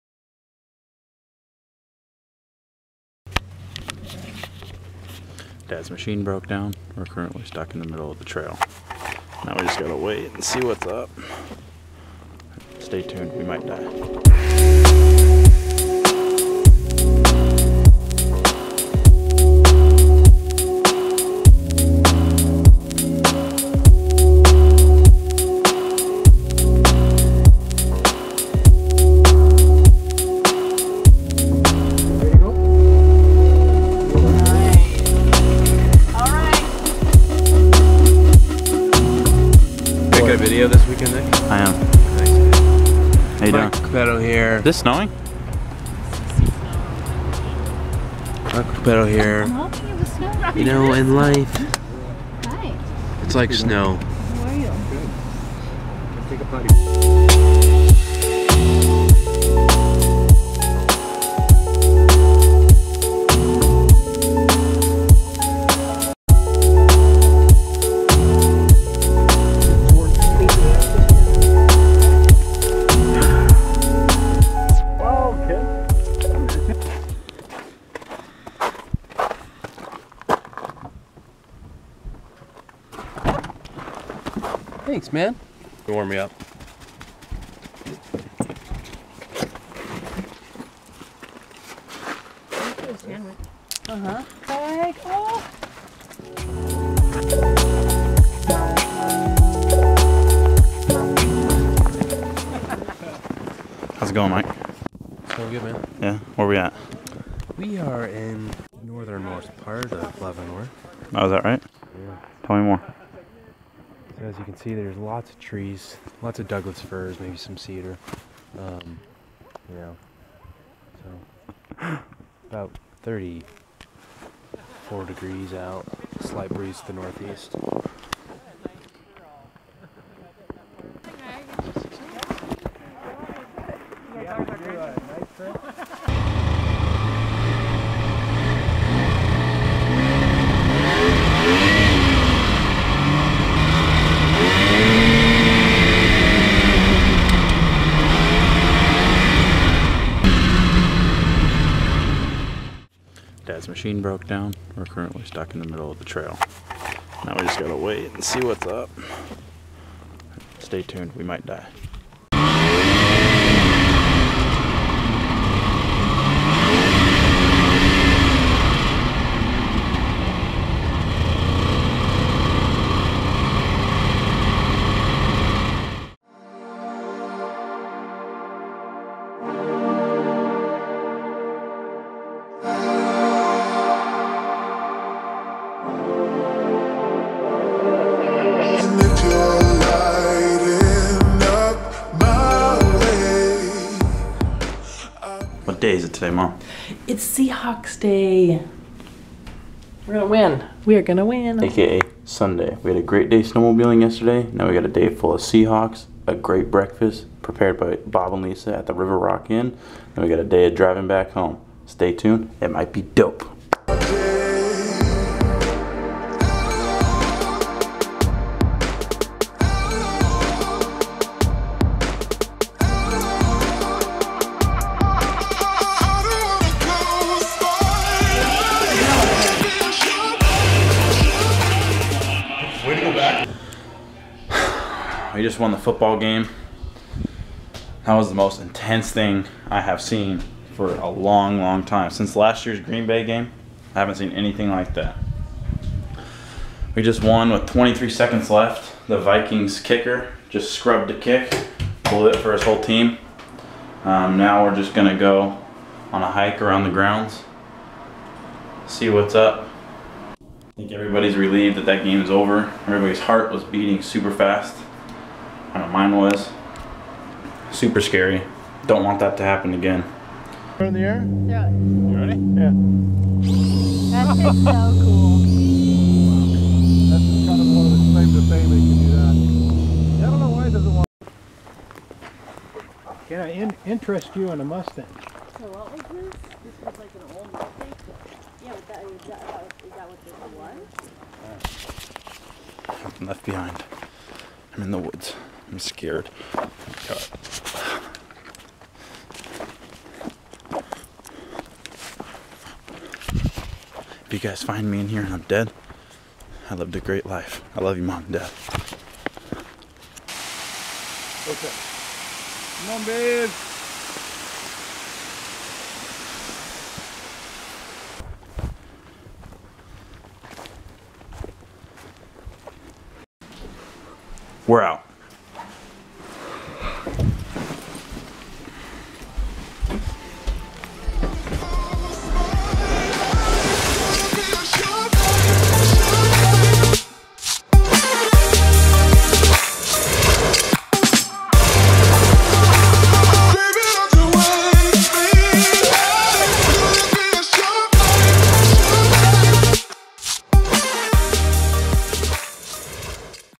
Throwback Family Vacation
I filmed this video last winter when my family and I took a trip to Leavenworth to snowmobile and stay at a bed and breakfast.
This was the first vlog I had ever filmed and am now just putting it out.
Hope you guys enjoyed the flashback.
thanks yall!